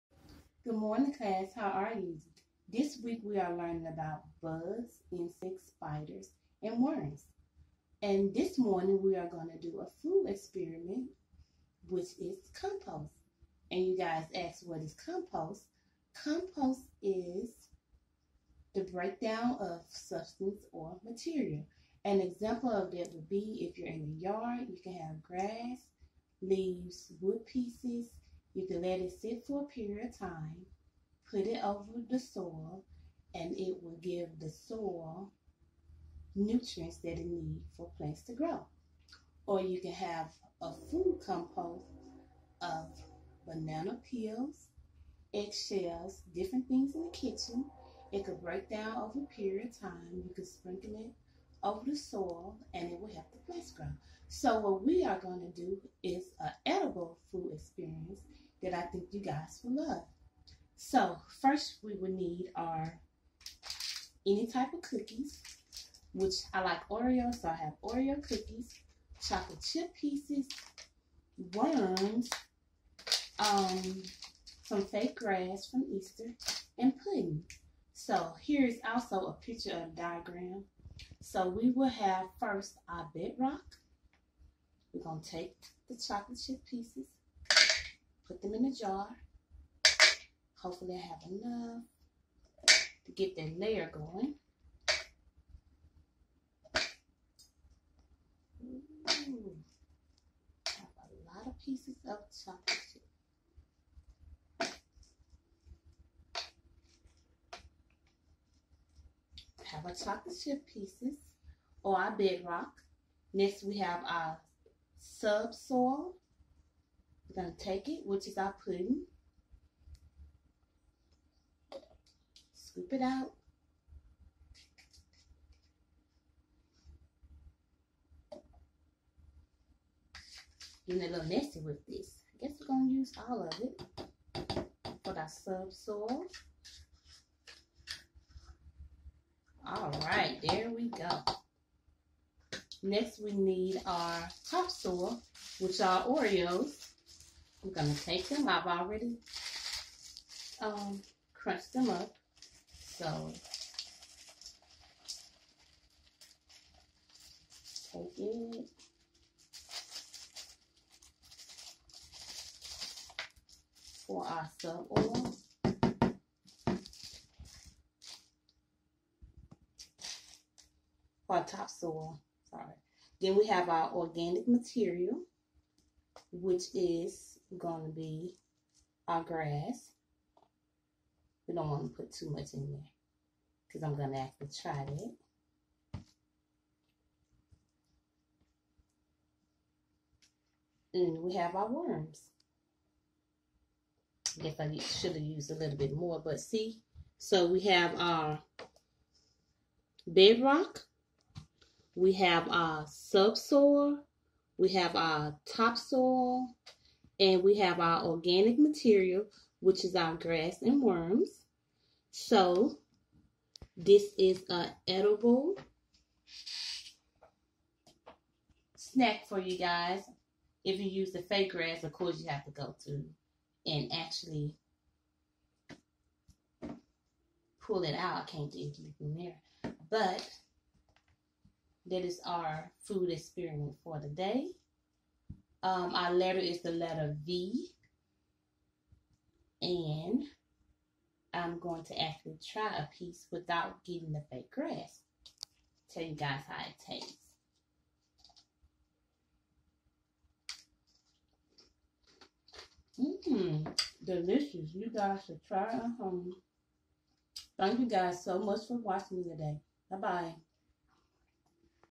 CADC Arkadelphia Head Start/ABC - Ms. Shree - Food Experience
Ms. Shree conducting an edible compost food experience with Oreo, pudding, chocolate chip, fake grass and gummy worms